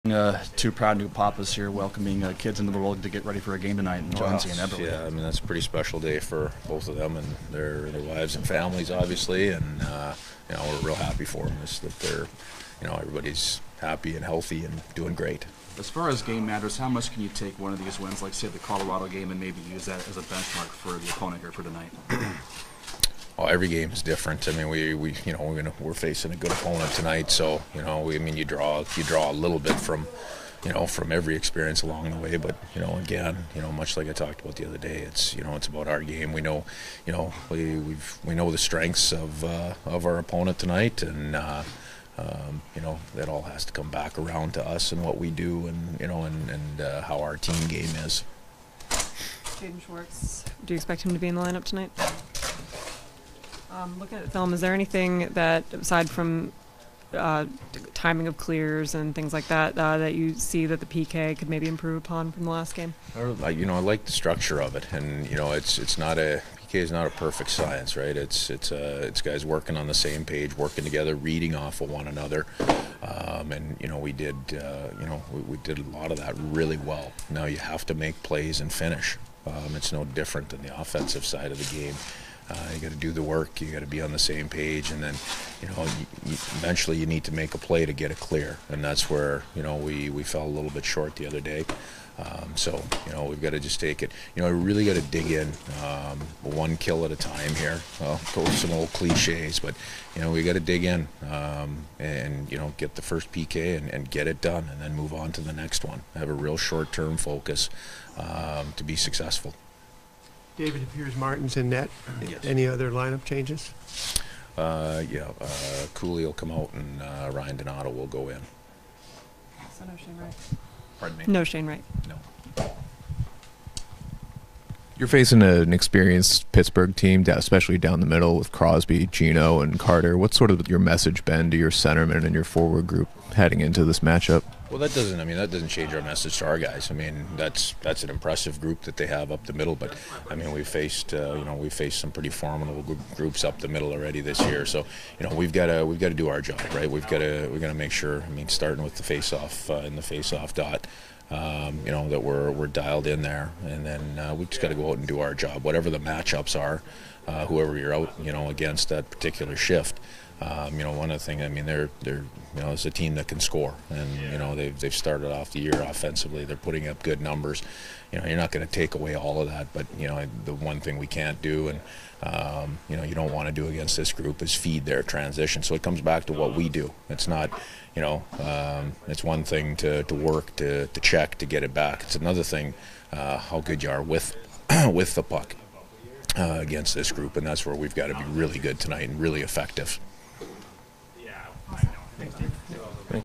Kraken Sound: Dave Hakstol - Oct. 29, 2022 Pregame
Coach Hakstol Talks to the media ahead of the Krakens game against  Pittsburgh.